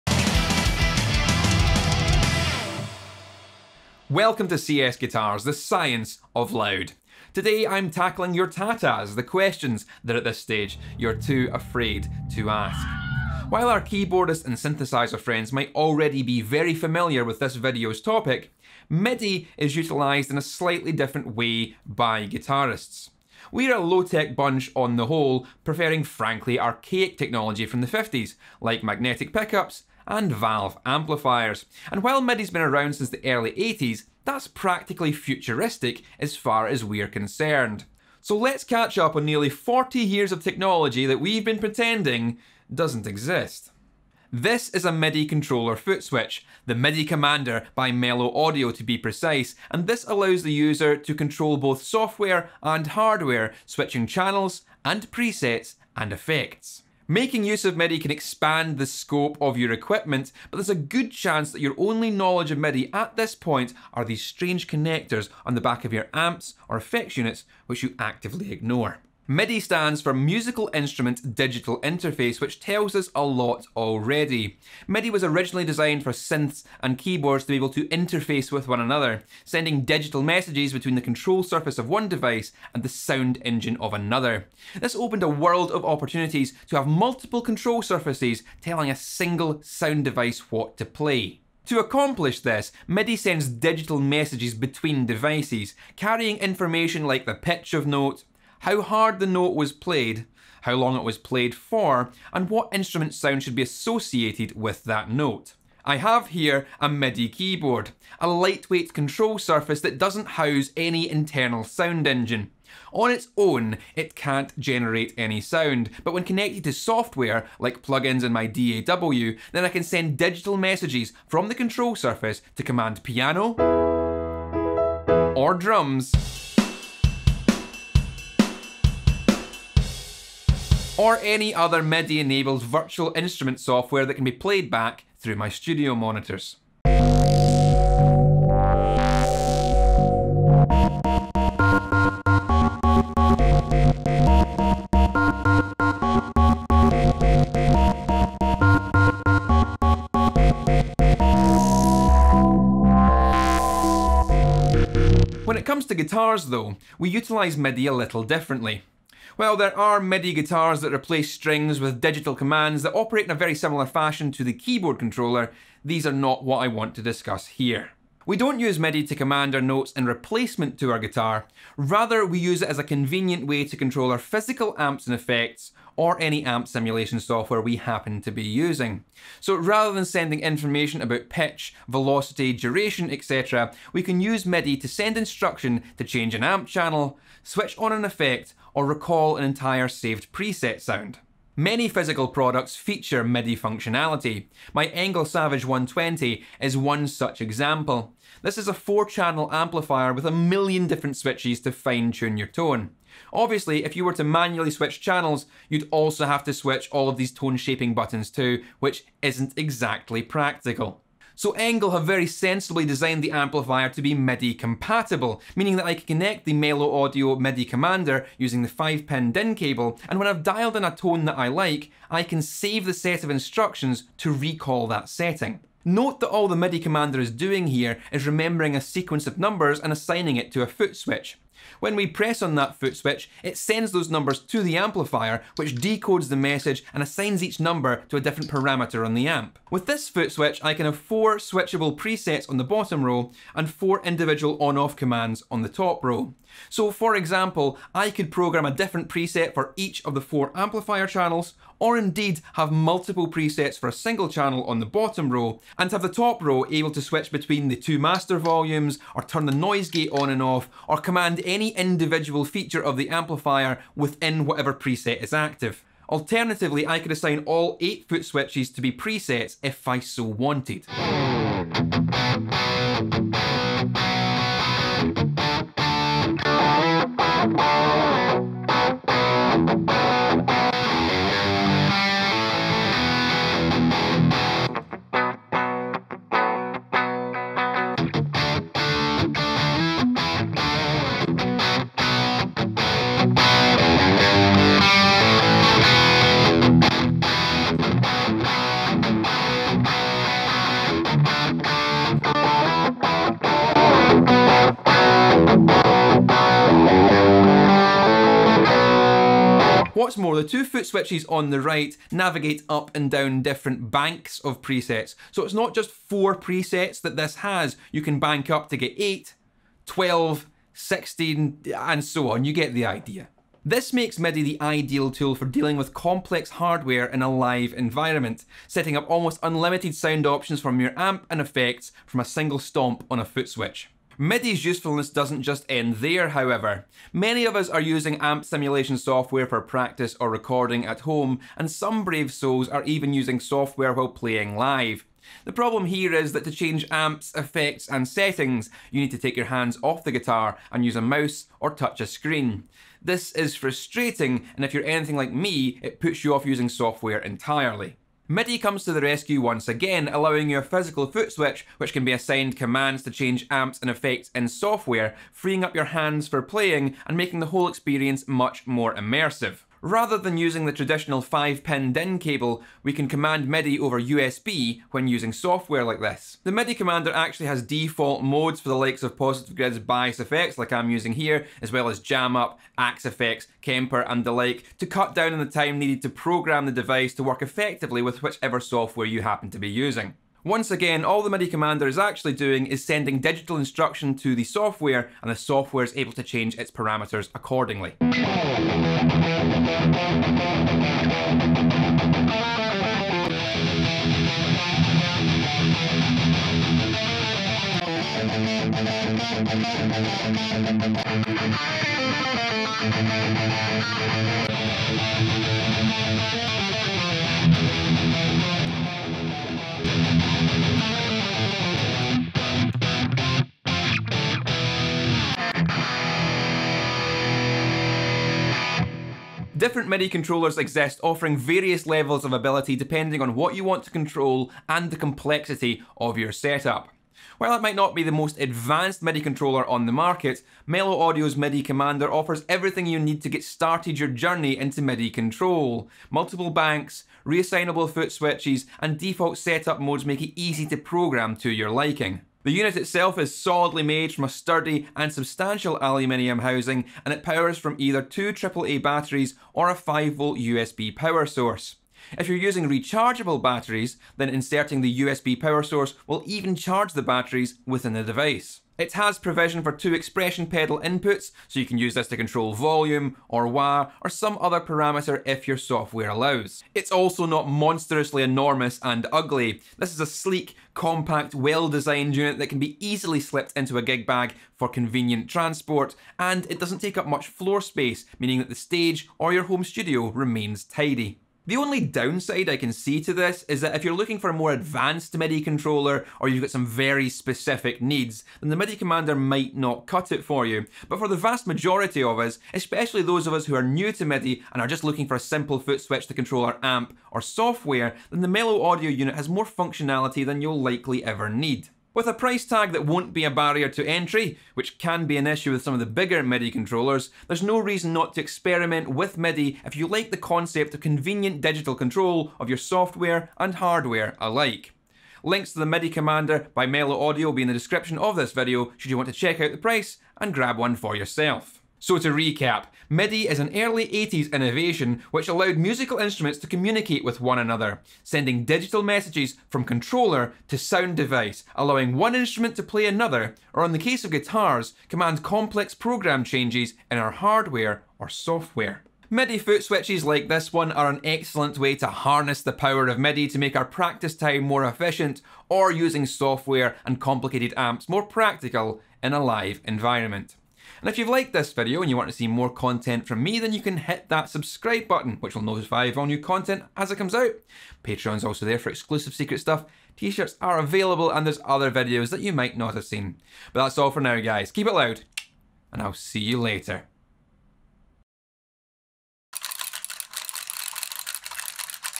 What is MIDI? An Introduction For Guitarists | Too Afraid To Ask [Meloaudio MIDI Commander]
MIDI is a powerful tool, allowing us to control complex hardware and software easily, but what is it?
We take a look at a MIDI controller footswitch to see how MIDI can be utilised for guitars in this Too Afraid To Ask video.

Leave your own TATAs in the comments section and perhaps they'll become the topic of a future video.

CSGuitars uses:
Rode Microphones: